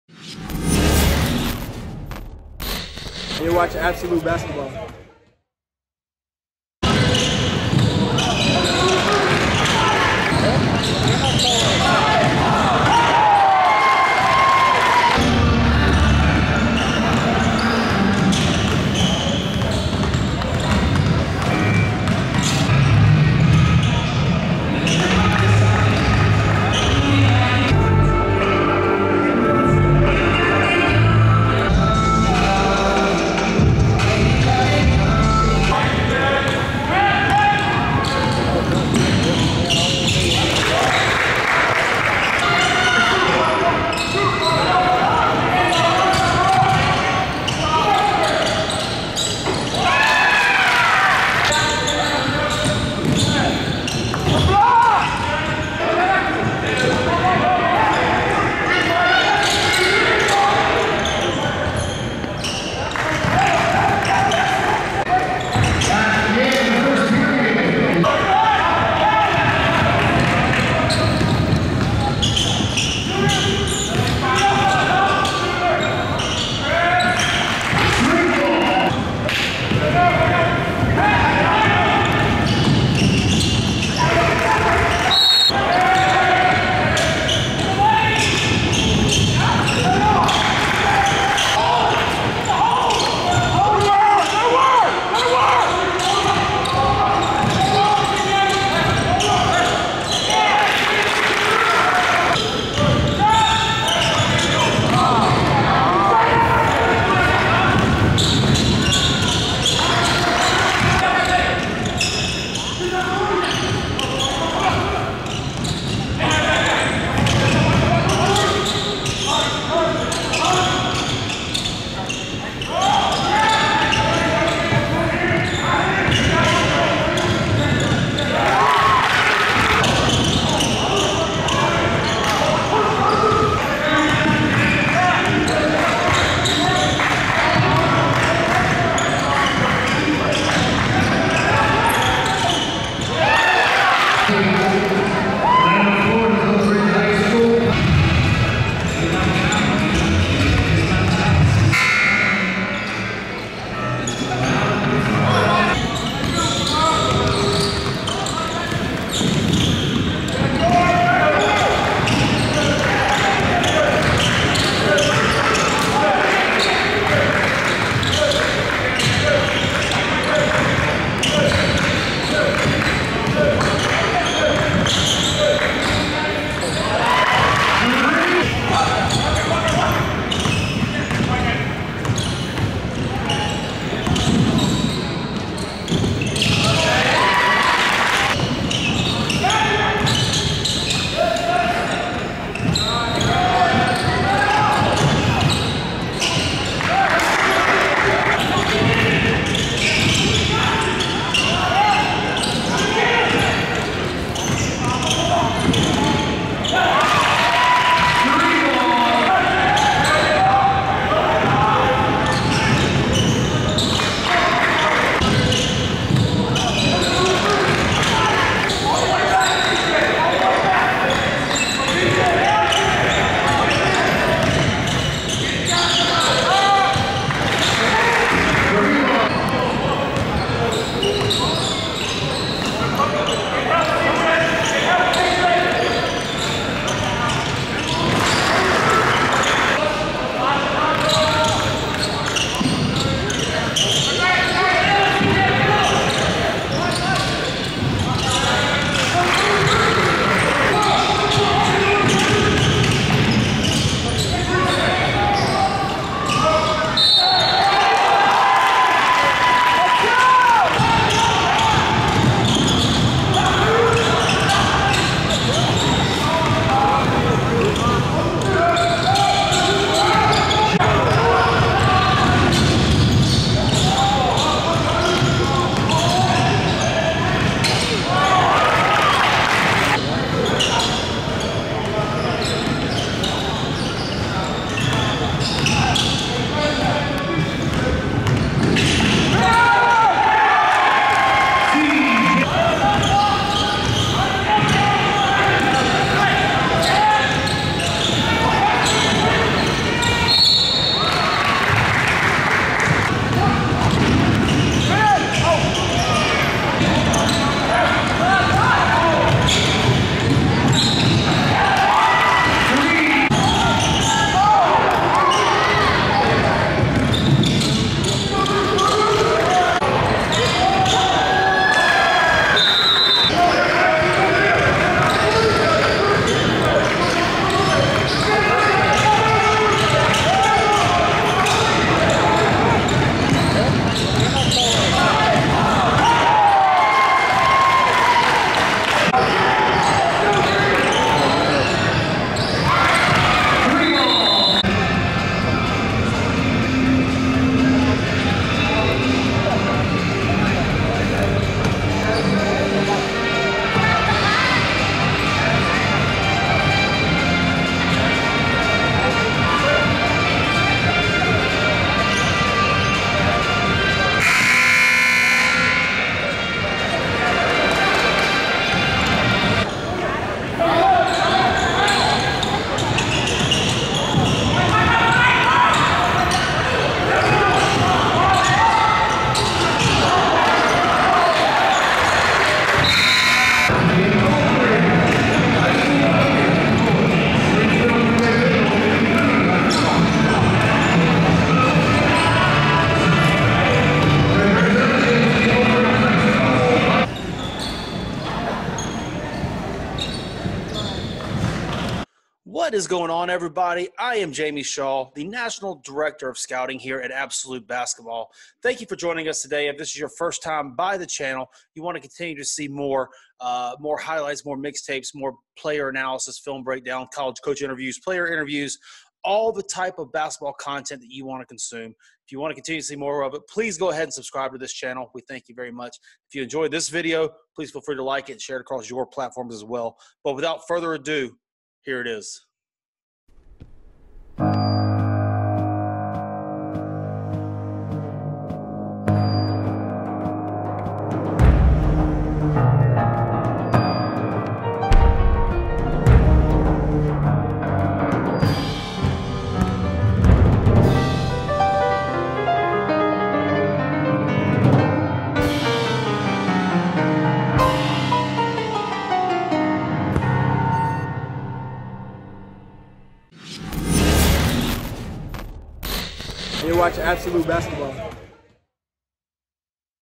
Louisville signee Mike James & SMU Signee Jalen Smith! Introducing Sophomore Jaydon Young!
Louisville signee Mike James & SMU Signee Jalen Smith led Oak Ridge. Introducing Sophomore Jaydon Young with Greensboro Day. This was a 3rd place game at the Beach Ball Classic.

Greensboro Day led for a lot of the game, but Mike James and Jaylon Smith caught their strides in the second half. Throughout, Jaydon Young was very good.

Also for Oak Ridge Fabio Basili made a name for himself while Tyler Lundblade and Michael Zanoni was very good for Greensboro Day

It was a game of runs, that went to the very end and we were there from start to finish to watch this one, all the highlights and all the exciting plays, and we documented it all for you ... ENJOY!!